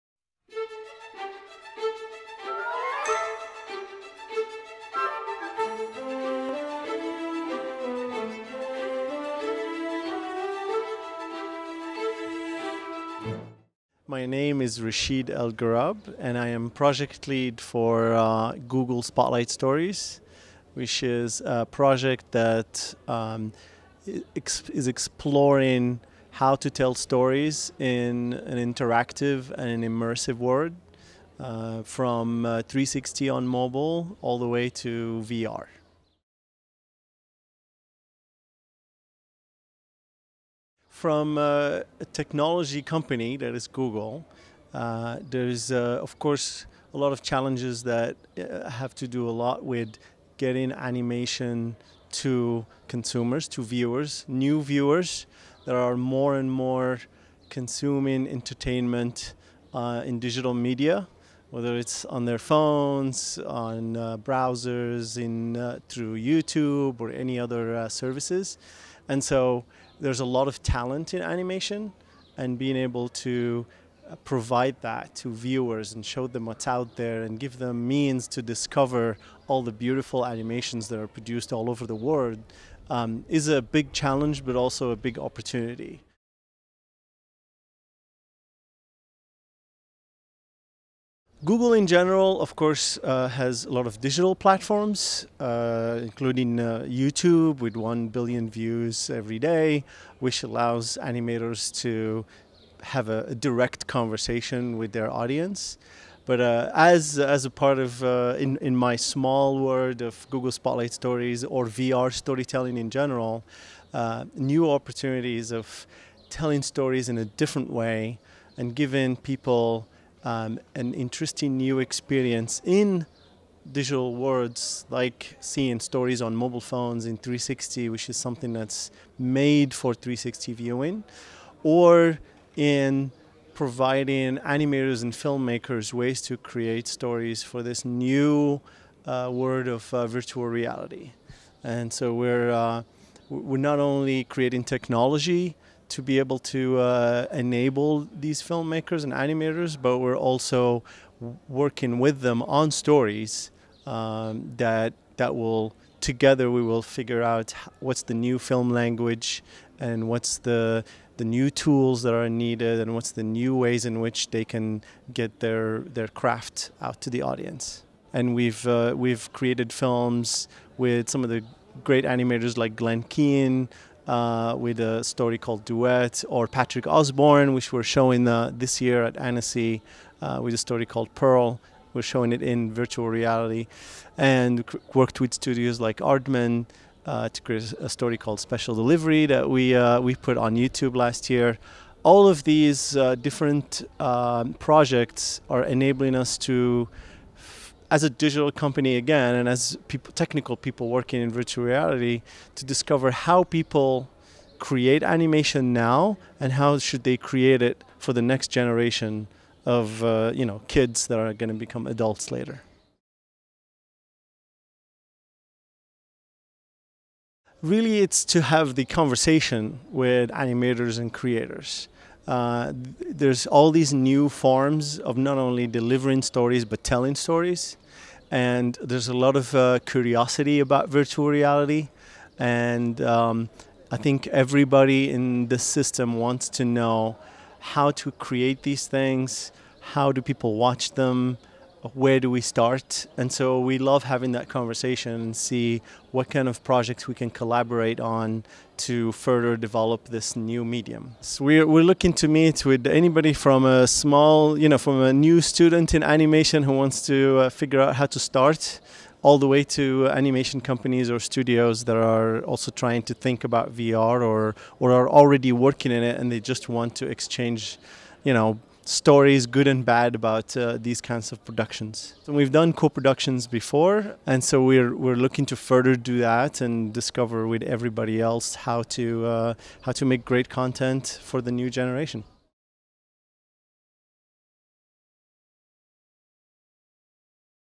Rachid El Guerrab - MIFA 2016
Interview of Rachid El Guerrab - Project Lead, Google Spotlight Stories - MIFA 2016

Google Life Story, a project exploring how to tell stories in an interactive and immersive world from 360 on mobile all the way to VR, was in Annecy this year looking for further collaborations and demoing its latest shows produced in partnership with creators or animation studios. "We’re not only creating technology […], we’re also working with creators on the stories, so that together we can figure out what’s the new film language […] and how to make great content for the next generations", says Project Lead, Rachid El Guerrab.

Google Life Story, un projet dont l’objectif est d’explorer les types de narration possibles pour les nouvelles technologies immersives et interactives que sont la réalité virtuelle et le 360°, était à Annecy pour rencontrer les créateurs et producteurs d’animation. Google Life Story montrait à Annecy ses dernières productions en réalité virtuelle, certaines coproduites avec des studios d’animation. "Nous ne faisons pas que créer la technologie […], nous travaillons également avec les créateurs sur les histoires, afin d’imaginer ensemble quels seront les langages de demain", commente le chef de projet Rachid El Guerrab.